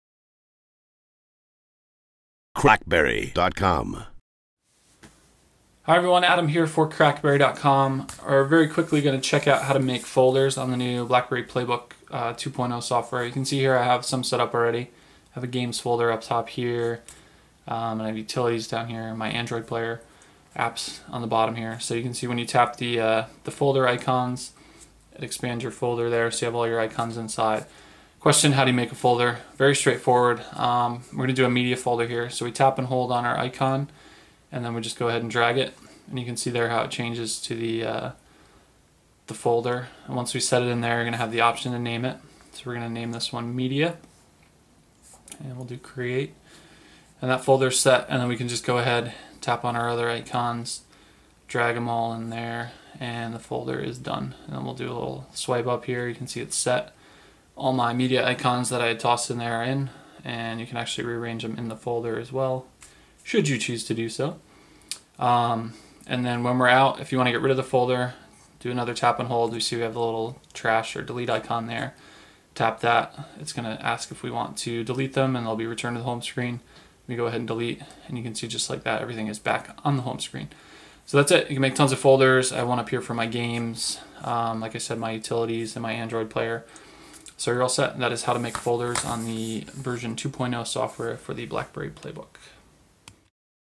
Creating app folders on the BlackBerry PlayBook
How to create app folders on the BlackBerry PlayBook 2.0 OS and BlackBerry 10 devices